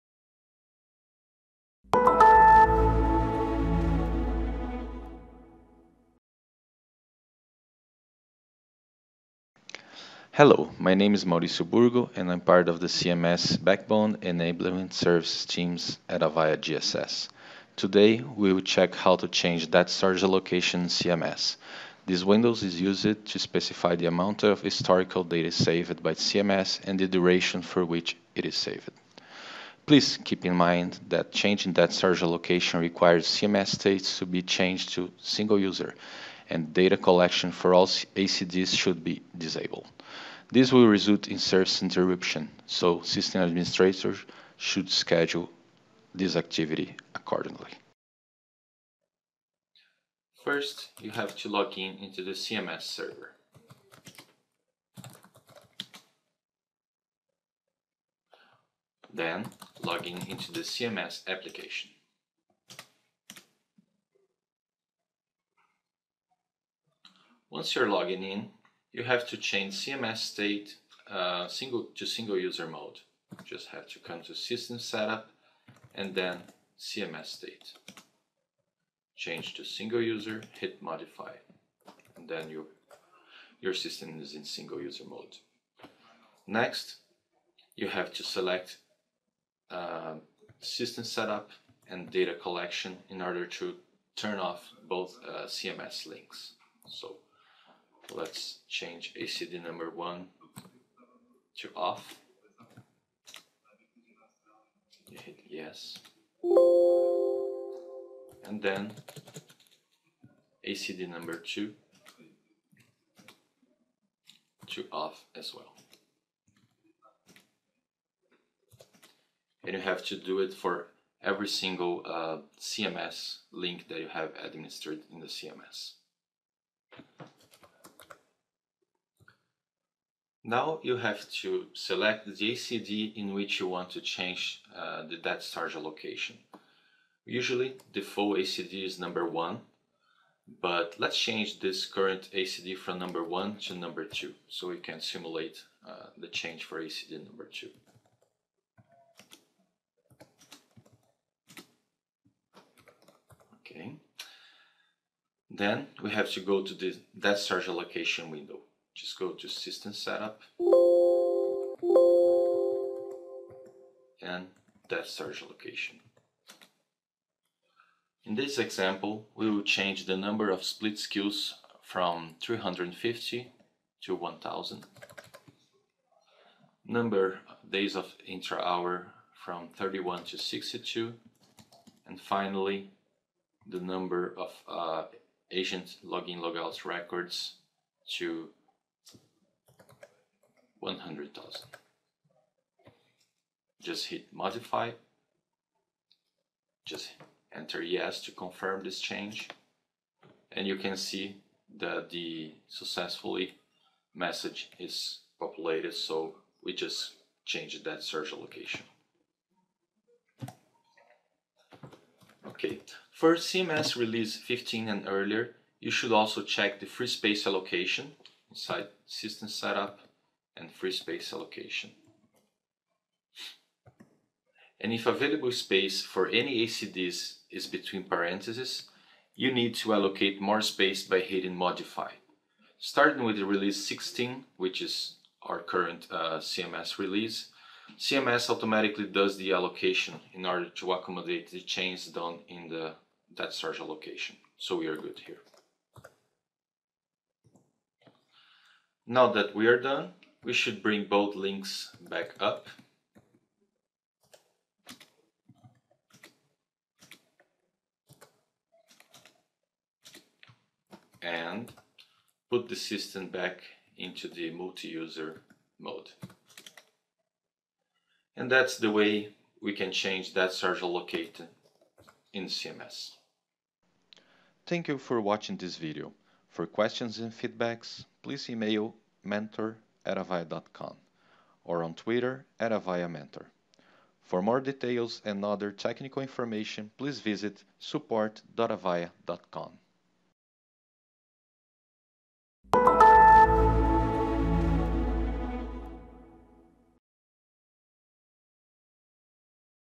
How to change data storage allocation in call management system
This video instructs on changing data storage allocation in CMS (Call Management System)
Produced by: Mauricio Burgo